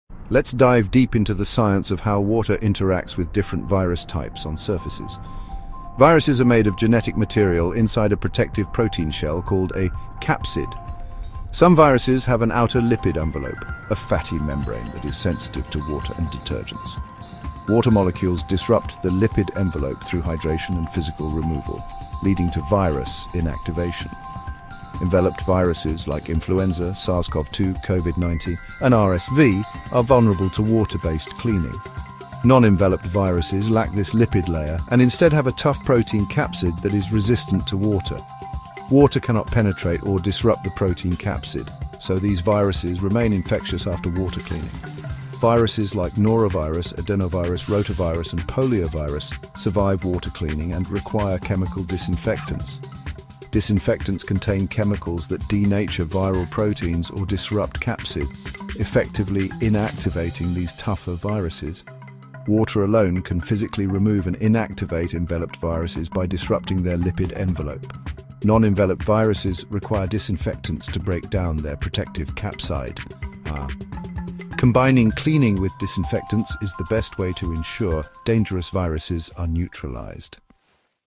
How to Make Viruses With Water?💧water
Can cleaning surfaces with just water remove dangerous viruses? 🤔
In this quick science explainer, learn why water can disrupt some viruses like flu and COVID-19, but why tougher viruses like norovirus need disinfectants to be eliminated.
Stay informed and keep your environment safe! 💧